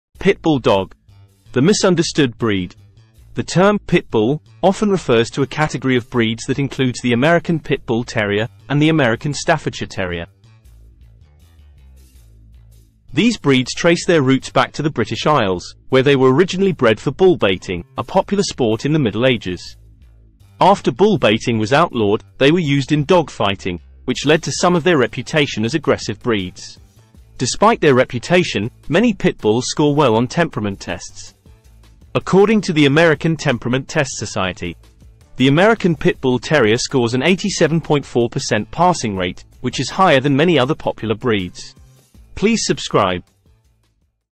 Pitbull Dog
The term "pit bull" often refers to a category of breeds that includes the American Pit Bull Terrier, the Staffordshire Bull Terrier, the American Staffordshire Terrier, and others. These breeds trace their roots back to the British Isles, where they were originally bred for bull-baiting, a popular sport in the Middle Ages. After bull-baiting was outlawed, they were used in dog fighting, which led to some of their reputation as aggressive breeds.

Despite their reputation, many pit bulls score well on temperament tests. According to the American Temperament Test Society, the American Pit Bull Terrier scores an 87.4% passing rate, which is higher than many other popular breeds.